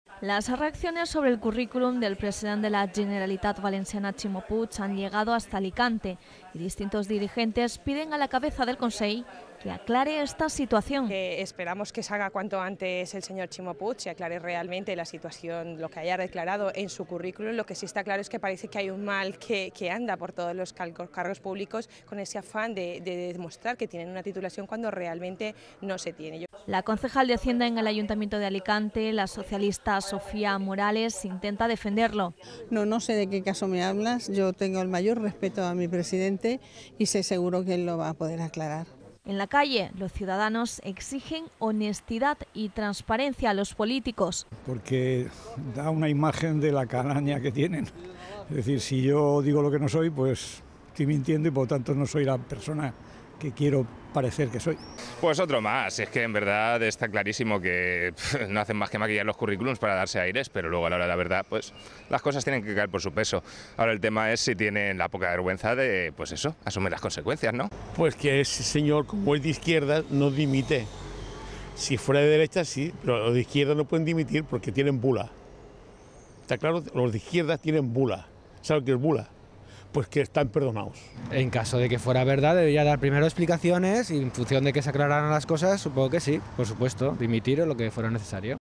Reacciones sobre el currÍculum del presidente Ximo Puig
Las reacciones sobre el currículum del presidente de la Generalidad Valenciana, Ximo Puig han llegado hasta Alicante y distintos dirigentes piden a la cabeza del Consell que aclare esta situación.